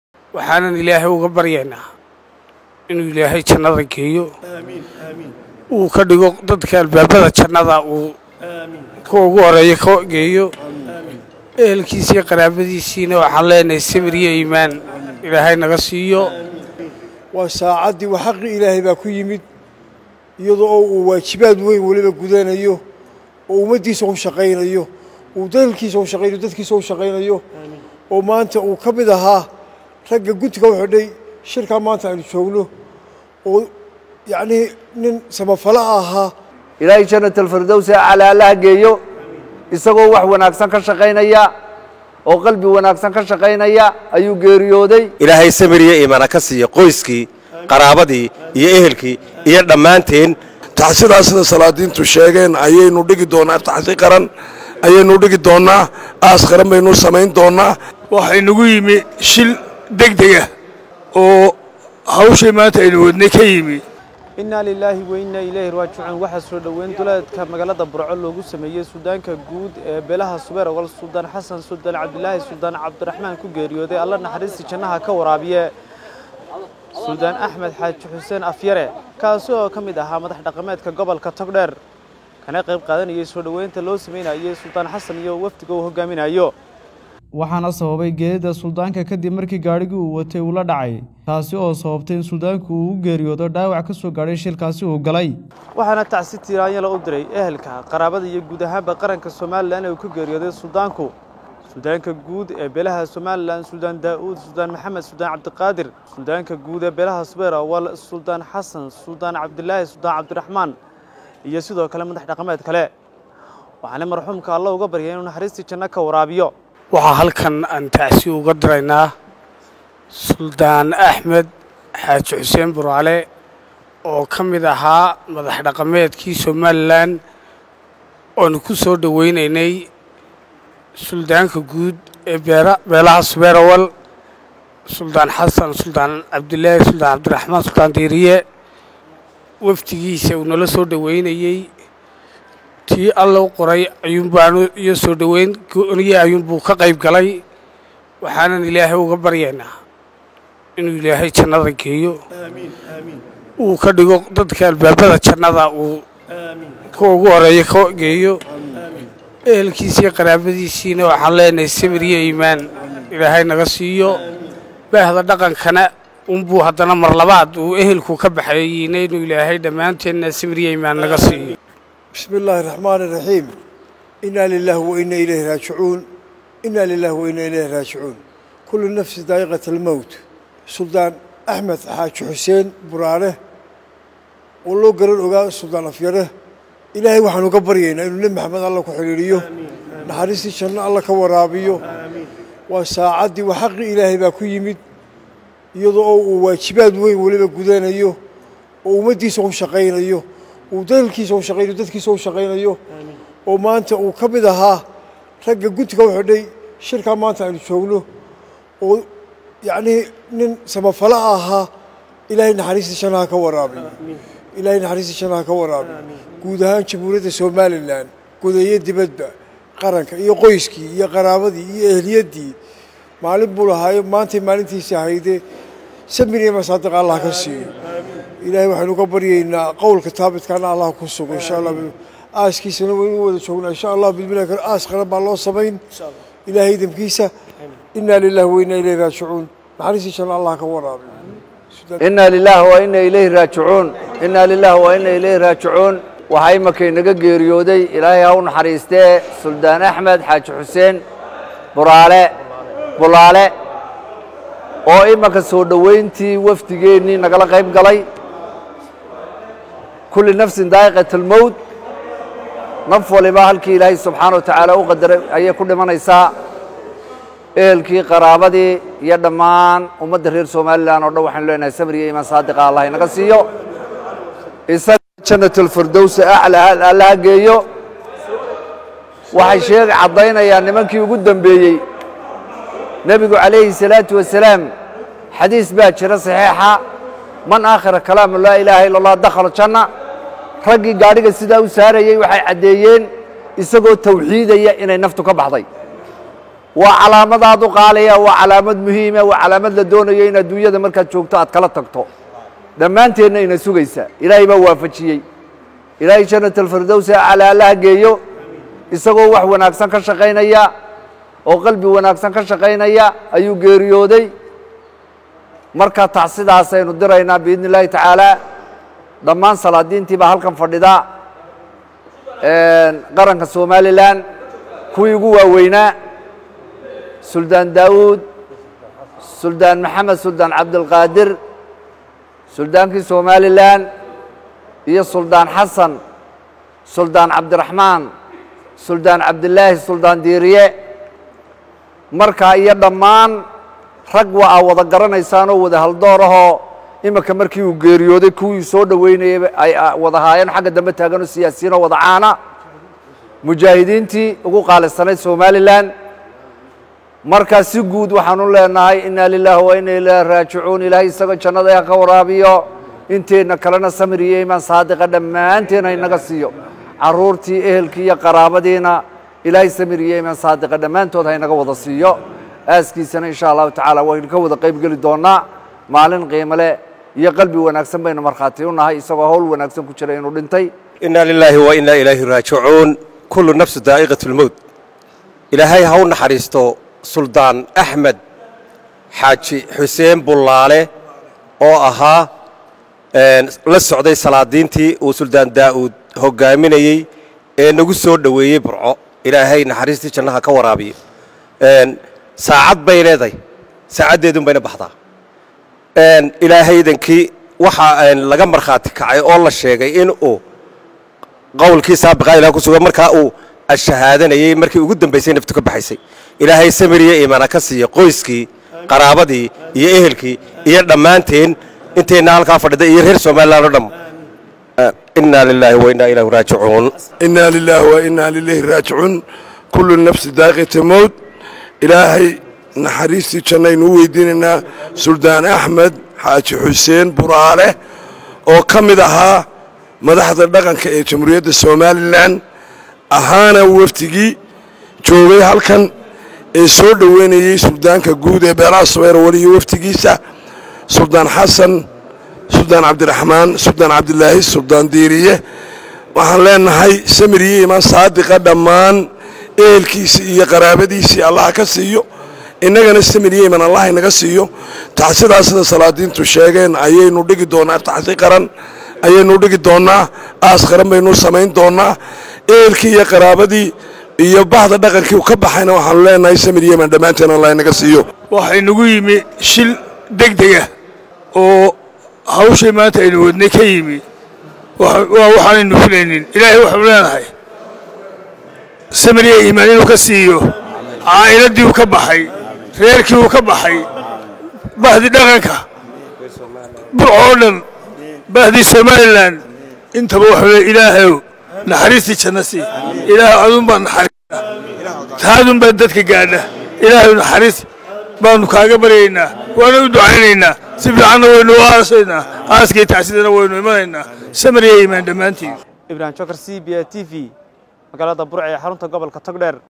Salaadiinta Somaliland ayaa tacsi u diray AHU Suldaan Afyare oo duleedka Burco gaadhi kula dhacay.
Salaadiinta Somaliland ayaa tacsi u diray AHU Suldaan Axmed Afyare oo goor dhawayd duleedka Burco gaadhi kula dhacay.